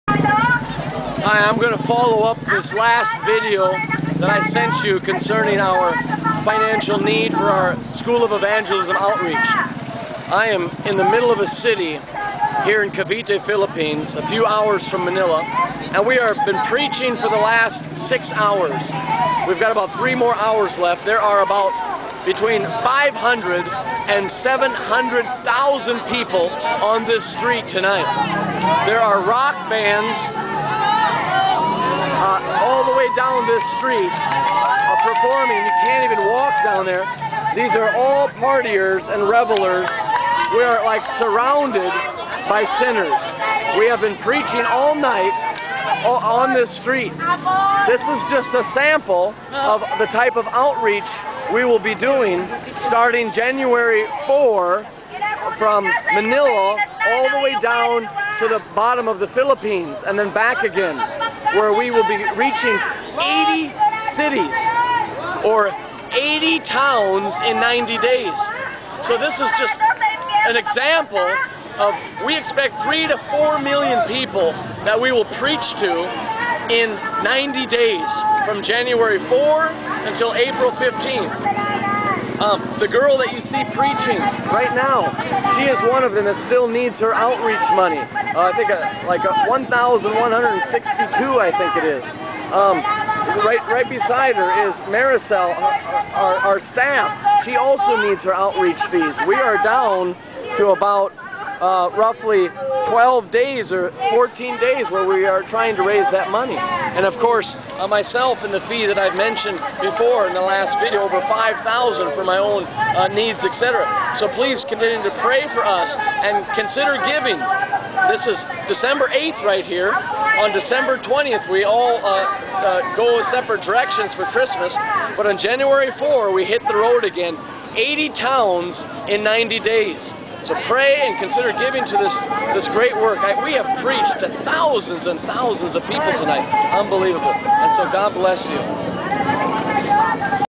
Fiesta of Revelers Pounded by Preachers
Listen to Mitch as he describes the sin and rebellion in the streets of Dasmarinas, Cavite, Philippines. Over 26 of us from the School of Evangelism and World Christian Fellowship "pounded" these "streets of sin" for nearly eight (8) hours last December 8th.